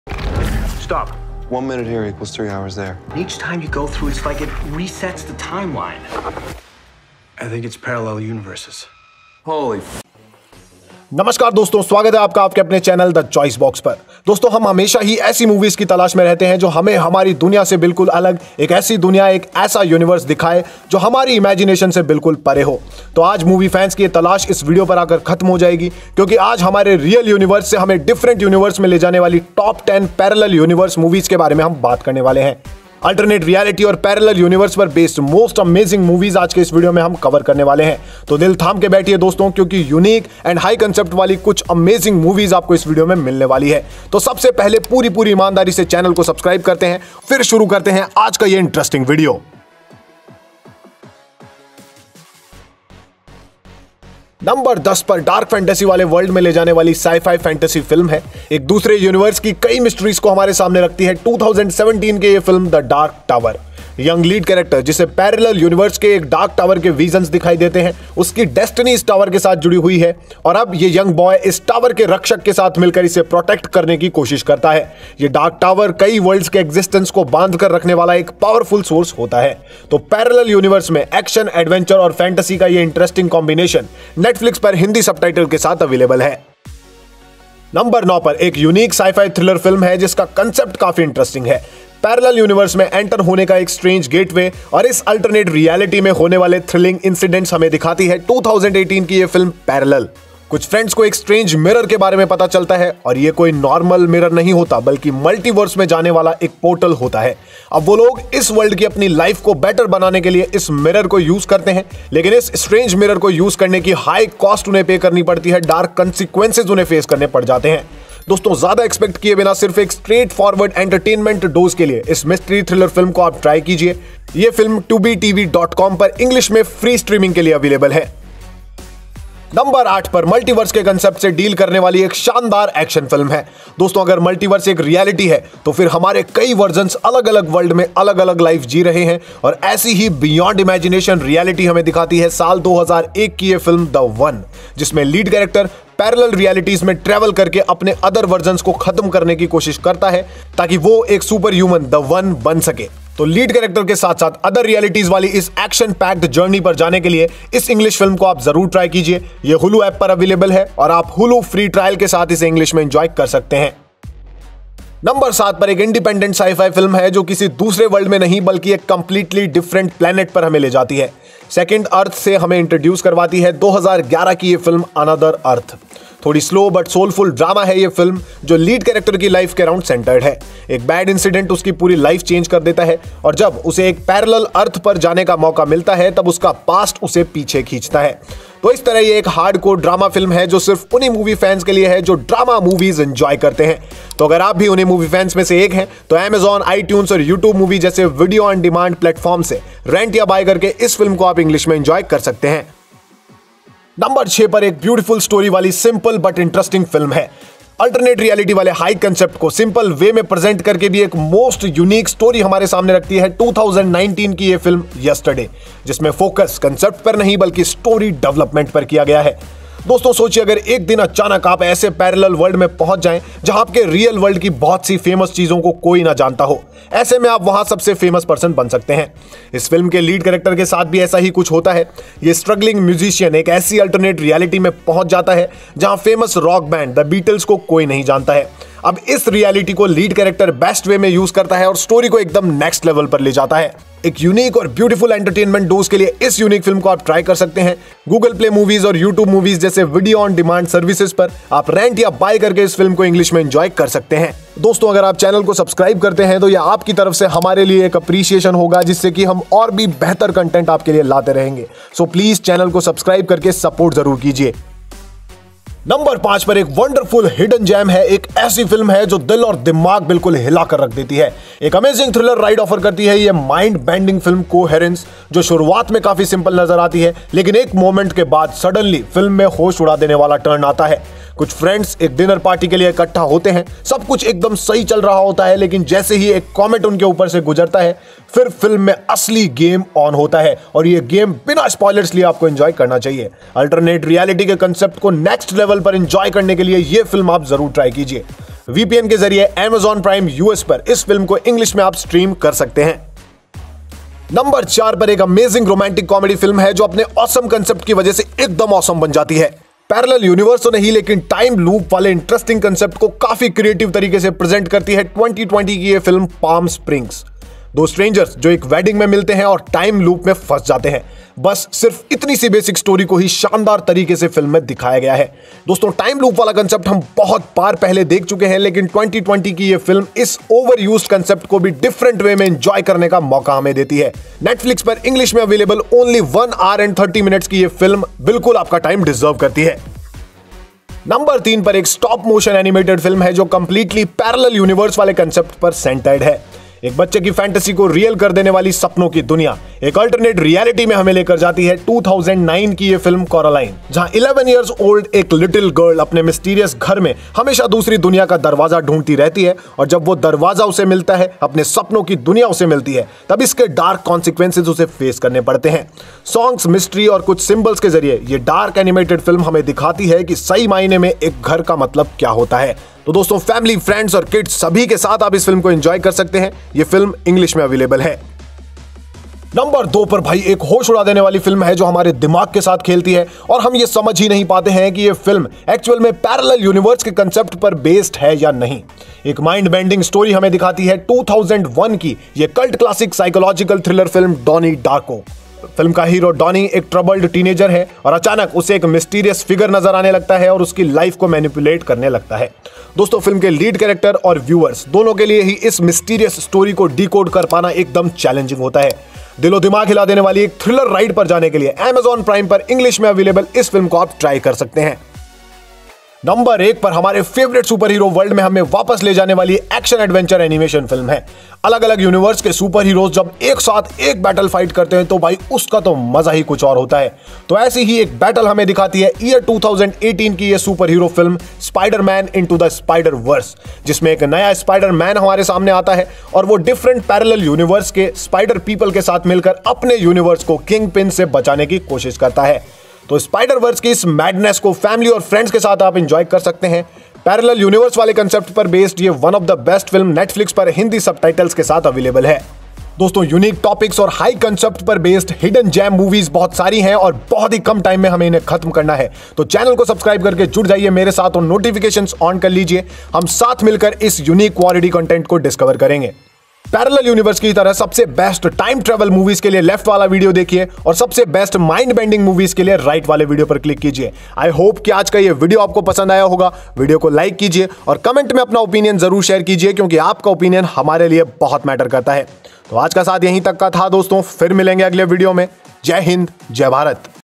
Top 10 Best Parallel Universe Movies (Multiverse😱) | Top 10 Best Alternate Reality Movies
These 10 Best Parallel Universe Movies Introduce us to the great concept of Multiverse and Alternate Reality.
We search the movies that can take us to another universe, an alternate universe that can make our fantasy a reality. This search ends here on this video beacuse we have listed top 10 best parallel universe movies or we can say the best alternate reality movies that will surely take you to the new world. Every moviefan should watch these movies with high concept.